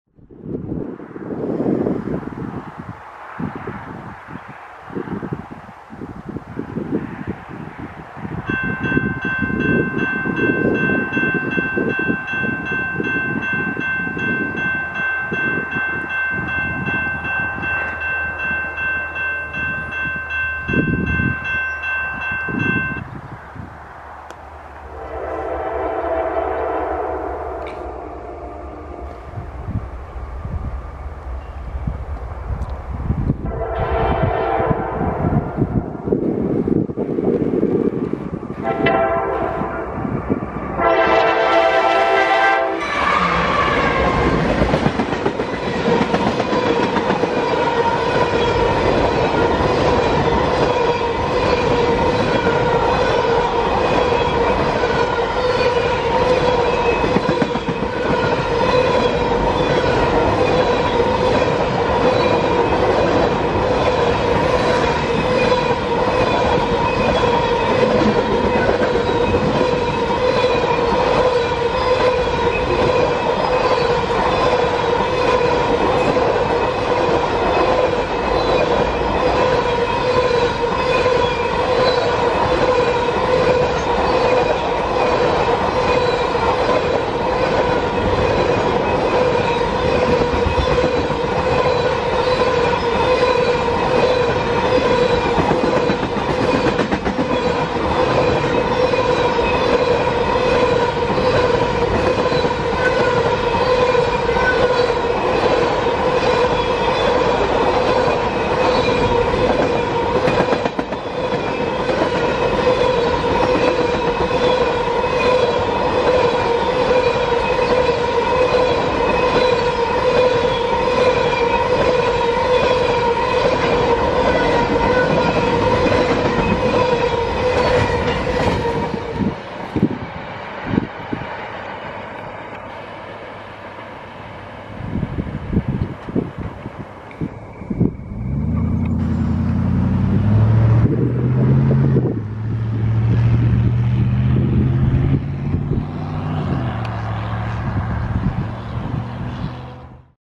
FAST CP ETHANOL TRAIN WITH NS 4284 AND NS 7592! CP Watertown Sub
Taken on 4/23/21 This day was a quick one since we had to do some other things and we couldn’t just railfan most of the day but I still managed to go to Columbus, WI quick and I heard a K5LA going by before we got there so we turned around And chased and found two NS locomotives we were able to chase it to the Salisbury Rd railroad crossing and the K5LA was amazing on the 681

Train Information NS 4284 NS 7952 Westbound 681 Ethanol Train 101 cars